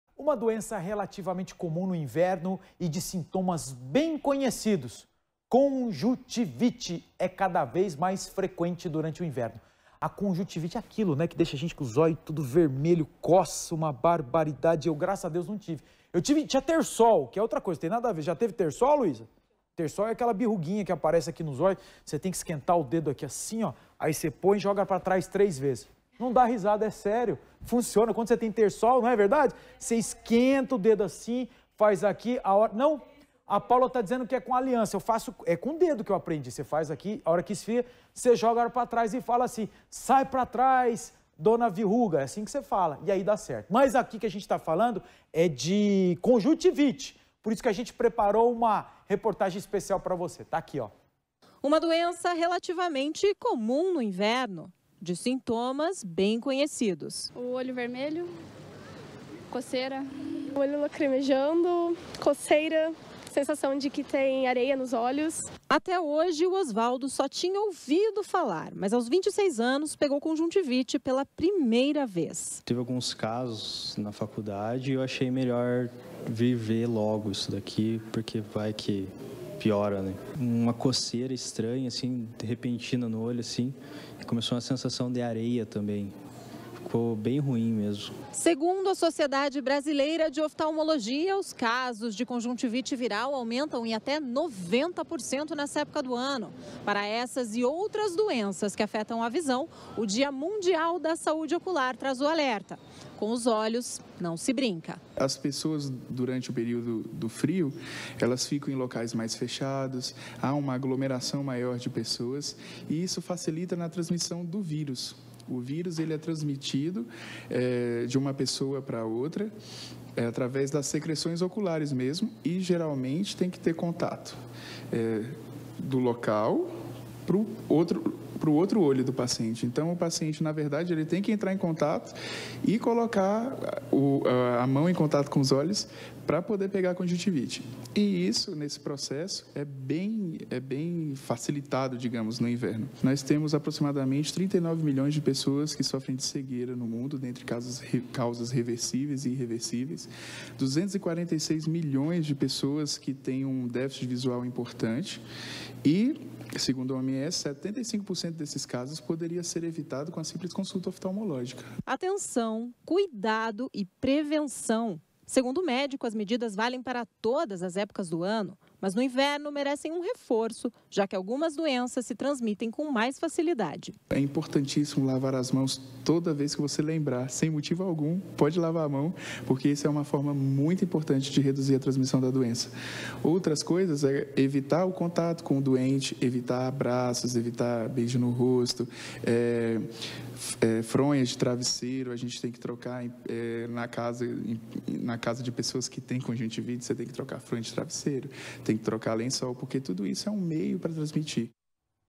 Saúde dos olhos: veja como evitar a conjuntivite no inverno
Com a chegada do frio, as pessoas ficam em locais mais fechados, isso facilita a transmissão de inúmeros vírus, entre eles a conjuntivite. Veja!